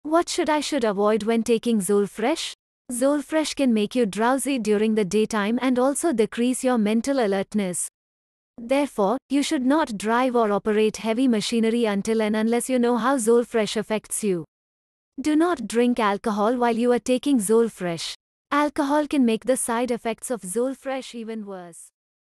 Q. What should I should avoid when taking Zolfresh?
Zolfresh 5 Tablet at  .
Zolfresh can make you drowsy during the daytime and also decrease your mental alertness. Therefore, you should not drive or operate heavy machinery until and unless you know how Zolfresh affects you. Do not drink alcohol while you are taking Zolfresh. Alcohol can make the side effects of Zolfresh even worse."